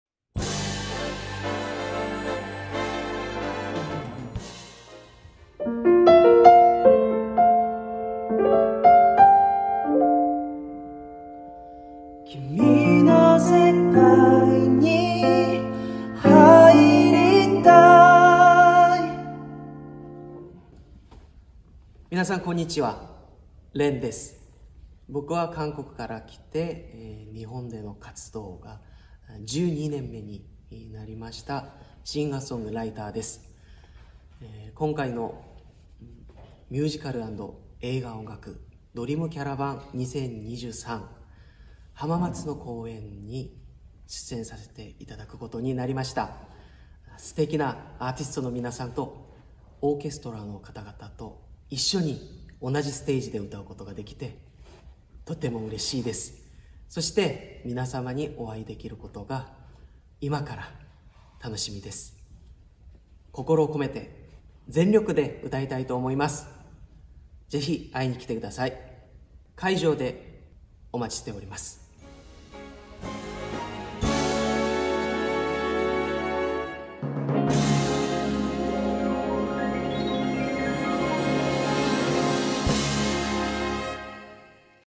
《LEN》メッセージ（ミュージカル＆映画音楽ドリーム・キャラバン2023）
☆2023年11月〜2024年1月、全国6都市にて開催！☆

ミュージカル＆映画音楽《ドリーム・キャラバン 2023》
with シンフォニック・ジャズ・オーケストラ

【東京公演】
　日時：2023年11月18日（土）開演18:00
　会場：東京オペラシティ コンサートホール

【大阪公演】
　日時：2023年12月12日（火）開演18:00
　会場：梅田芸術劇場 メインホール

【浜松公演】
　日時：2023年12月25日（月）開演18:00
　会場：アクトシティ浜松 大ホール

【新潟公演】
　日時：2024年1月14日（日）開演17:30
　会場：りゅーとぴあ 新潟市民芸術文化会館 コンサートホール

【川越公演】
　日時：2024年1月23日（火）開演18:00
　会場：ウェスタ川越 大ホール

【岡山公演】
　日時：2024年1月29日（月）開演18:00
　会場：岡山シンフォニーホール 大ホール

◉出演（［　］内は出演する公演地）※五十音順
【ヴォーカリスト】
　朝夏まなと［大阪、川越］
　上原理生［東京］
　浦井健治［新潟］
　小池徹平［大阪］
　小柳ゆき［新潟］
　佐藤隆紀［川越］
　サラ・オレイン［大阪］
　堂珍嘉邦［川越］
　當間ローズ（MC兼）［東京、大阪、浜松、新潟］
　中川晃教［岡山］
　新妻聖子［東京、新潟］
　新納慎也［大阪、浜松］
　野口五郎［浜松、川越、岡山］
　はいだしょうこ［浜松］
　濱田めぐみ［東京］
　日野真一郎［東京、岡山］
　森口博子（MC兼）［川越、岡山］
　森崎ウィン［新潟］
　LEN［浜松］
　渡辺大輔［岡山］

【スペシャル・ソリスト】
　寺井尚子（Violin）［全公演］

【オーケストラ】
　千葉交響楽団［東京］
　岡山フィルハーモニック管弦楽団［大阪］
　中部フィルハーモニー交響楽団［浜松］
　東京交響楽団［新潟］
　群馬交響楽団［川越］
　関西フィルハーモニー管弦楽団［岡山］

【ビッグバンド】
　宮間利之ニューハード［東京、新潟、川越］
　アロージャズオーケストラ［大阪、浜松、岡山］

【指揮】
　河合尚市［全公演］